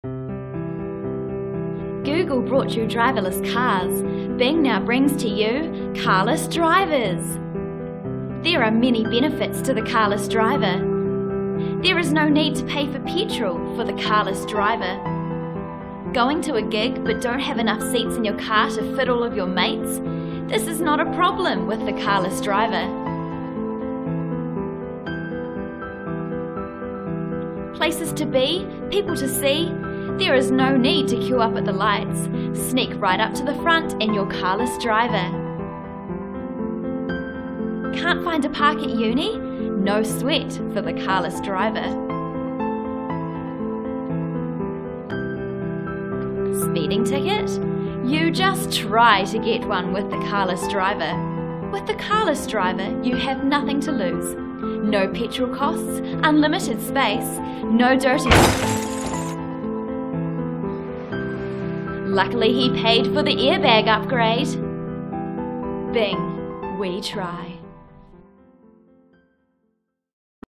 Bing Carless Drivers
Video Sketches from the 2015 OUSA Capping Show, CAMPUS WATCH.